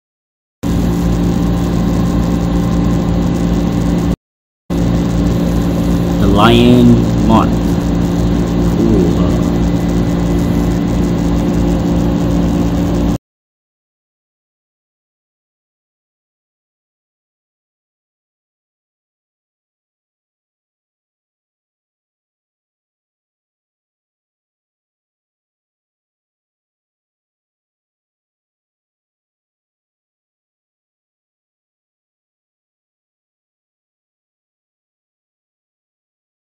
The Lion Faced Moth
This moth landed next to me at my work.  It looked like any other moth but when it flapped it's wings...WOW!  Very, very cool.